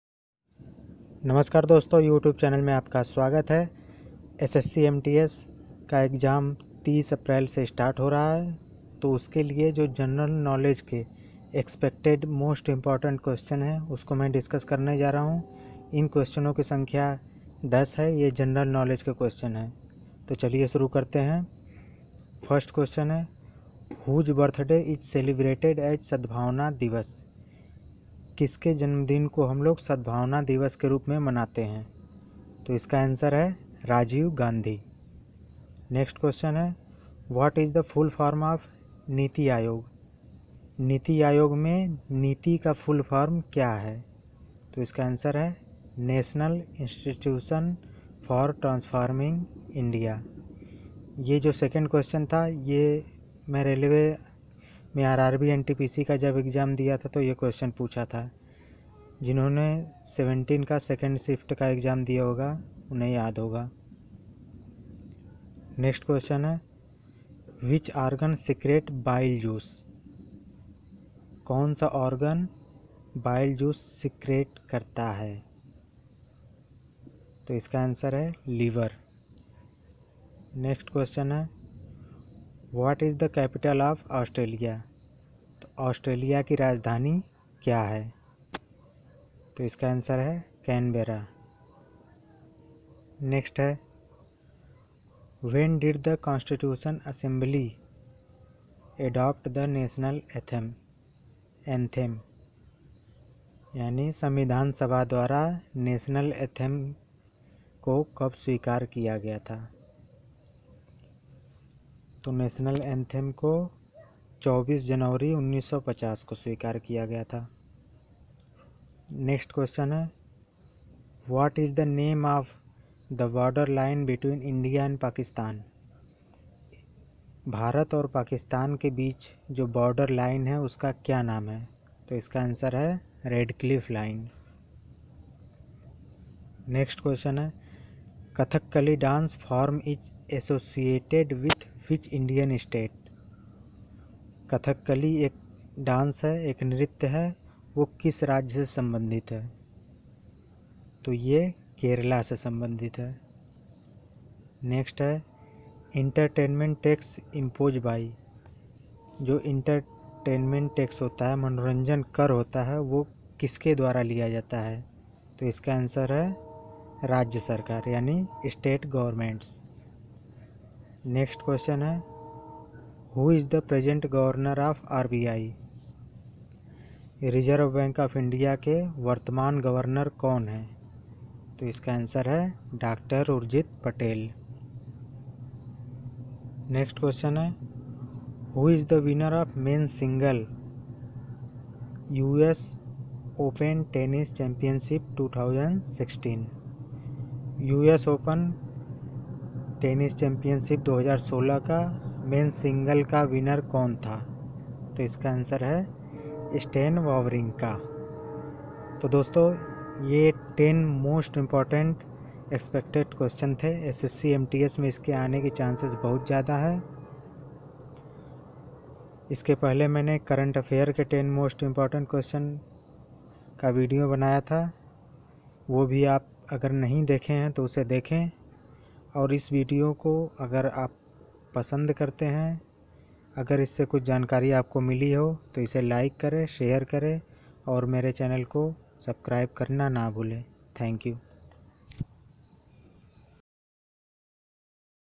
Most Important Expected General Knowledge Questions For SSC MTS 2017 | GK One Liner
SSC MTSGeneral Knowledgequestions asked in the  Exam will be designed to test the ability of the candidate’s general awareness of the environment around him and its application to society.SSC MTSGeneral Knowledge questionswill also be designed to test knowledge of current events and of suchmatters of everyday observation and experience in their scientific aspects as may be expected of an educated person.The test will also include questions relating to India and its neighbouring countries especially pertaining to Sports,History, Culture, Geography, Economic scene, General Polity including Indian Constitution, and Scientific Research etc. These questions will be such that they do not require a special study of any discipline.
Most Important Expecting general Knowledge Questions For SSC MTS 2017 One Liner.. After watching this video please comment your review friends...!